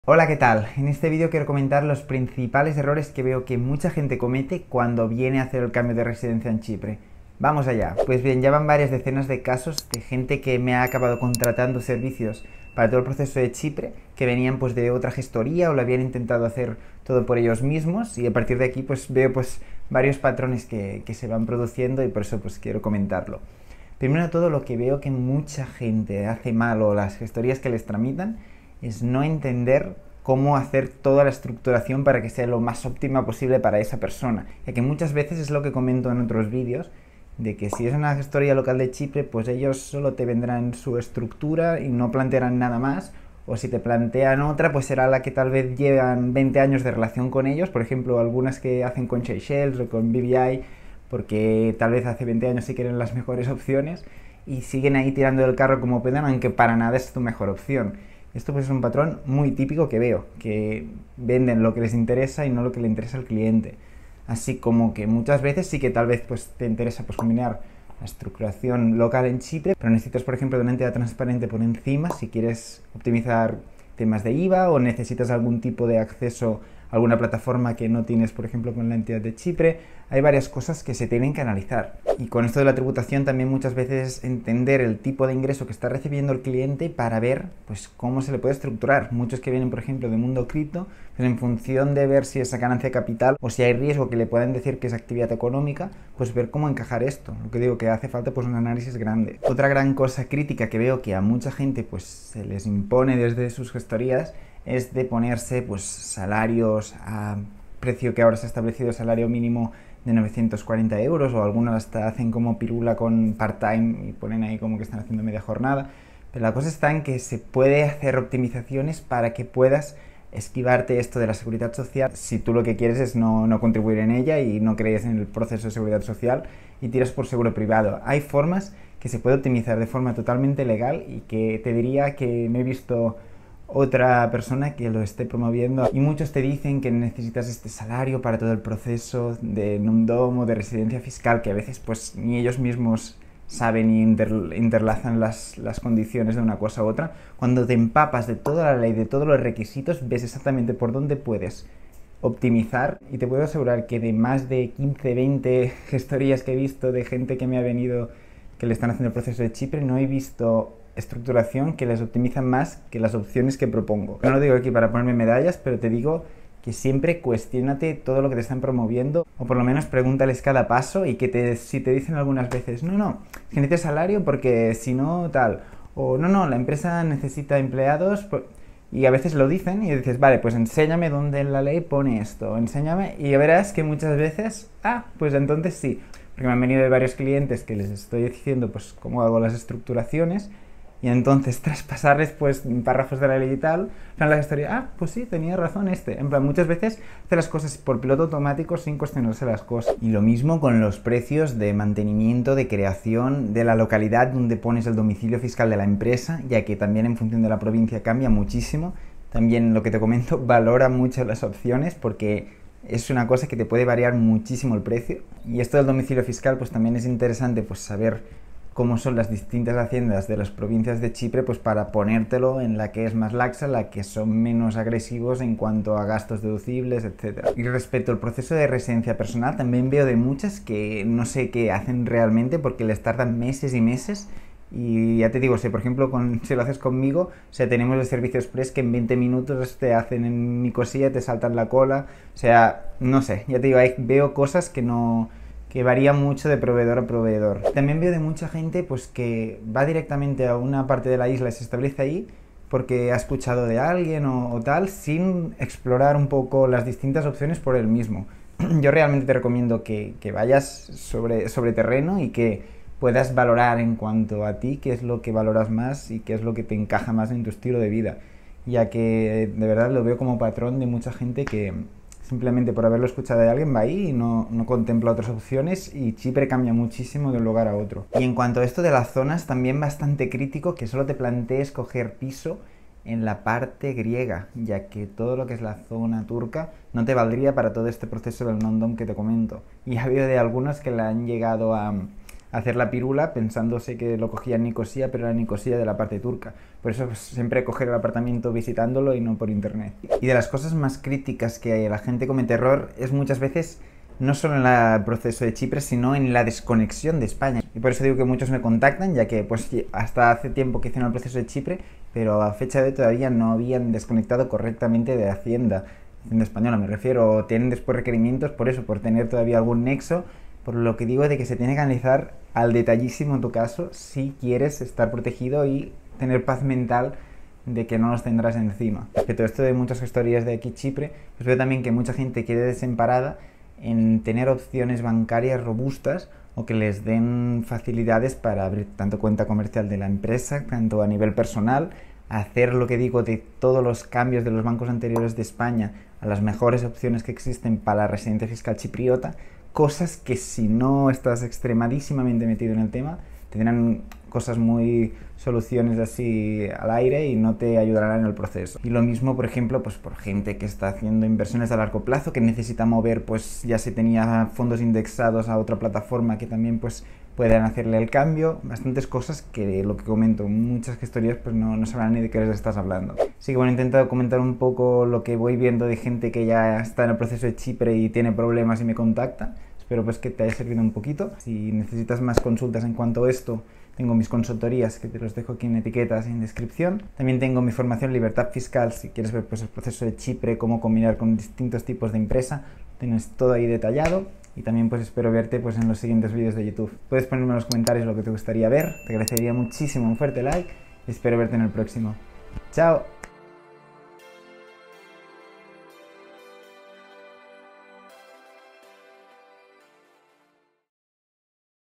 ⚠️RESIDENCIA FISCAL CHIPRE y Impuestos Non Dom ¡Errores CRÍTICOS! Vivir en Chipre🇨🇾
ERRORES CRÍTICOS al Cambiar la Residencia Fiscal a Chipre creando empresa en Chipre y aplicando a los impuestos non-dom.
📕 Mi Curso de Bolsa (25 Lecciones GRATIS)